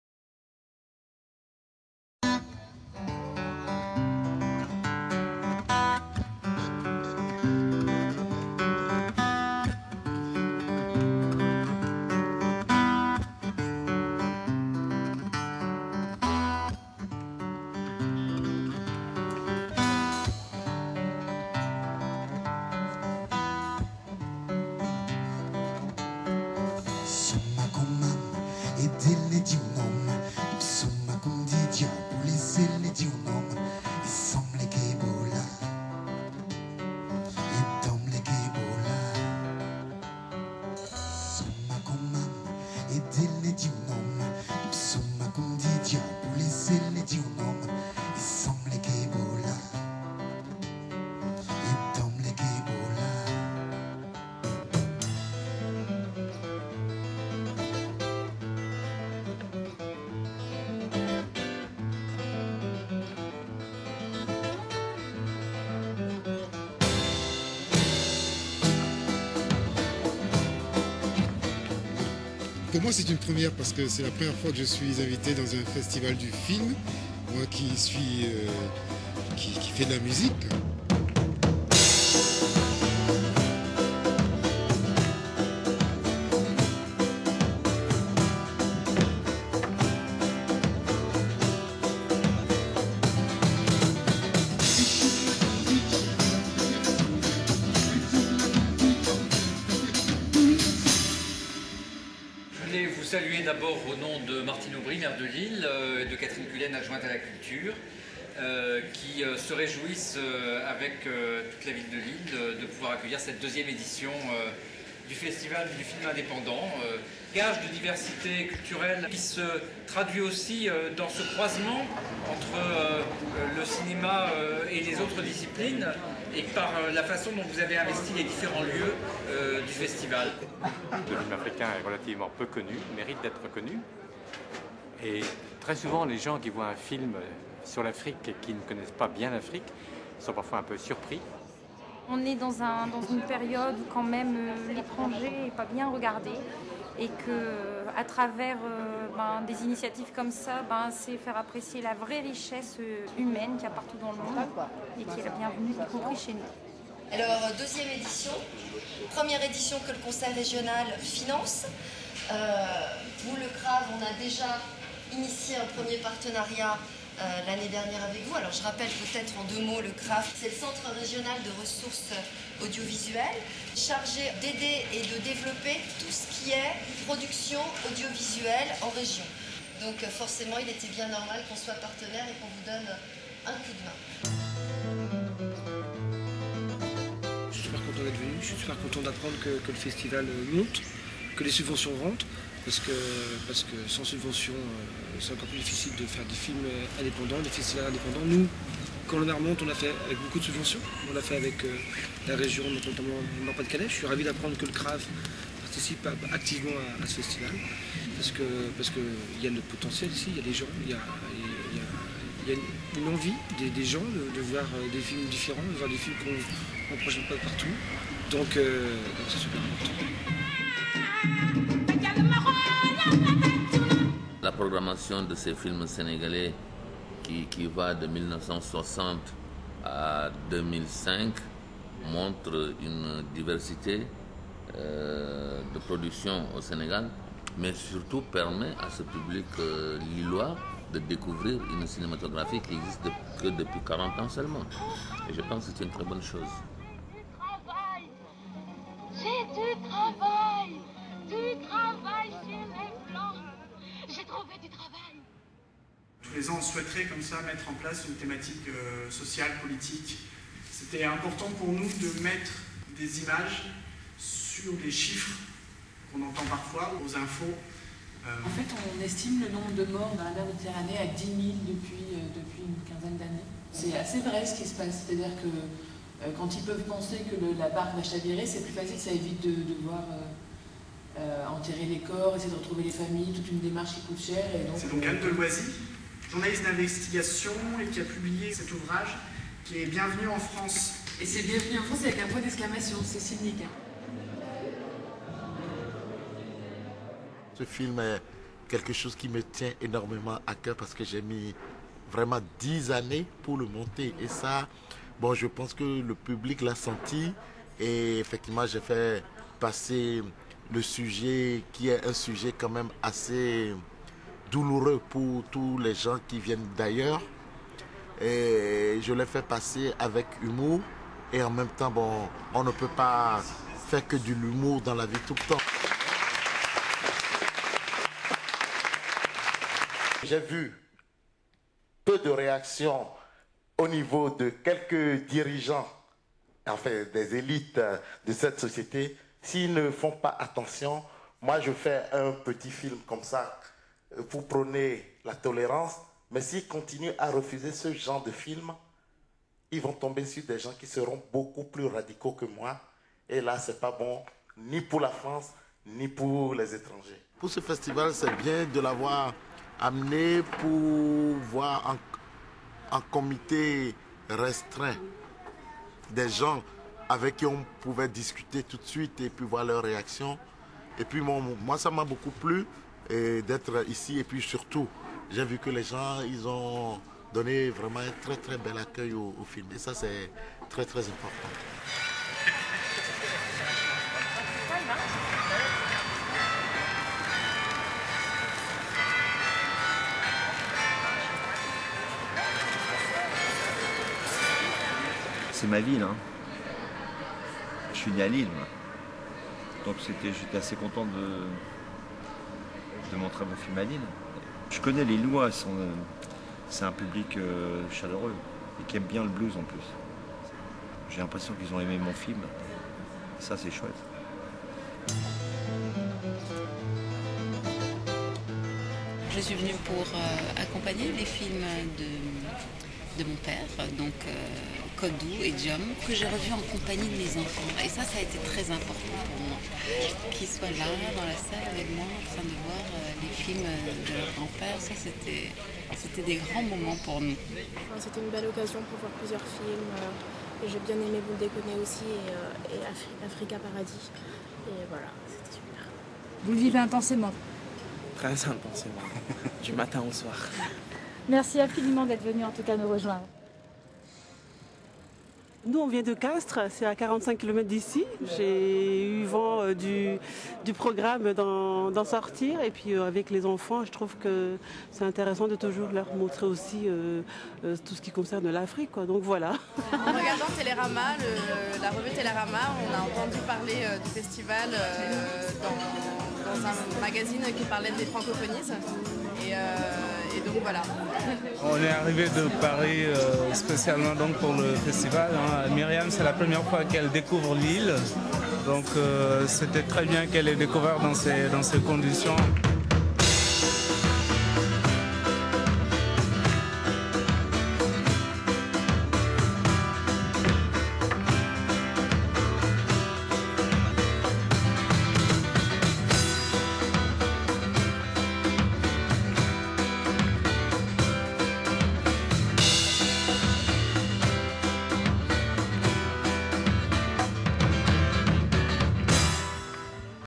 Festival International du Film Independant 2006 (#2)
Video de la 2ème édition du Festival International du Film Indépendant de Lille, en 2006.
Retrospective : Le Sénégal.
Invités : Dany LAFERRIERE, Richard BOHRINGER, Jacques Doillon, Thérèse M'Bissine Diop,
Abde Keta, Gilles Porte, Dominique Brenguier, Robinson Stevenin, etc.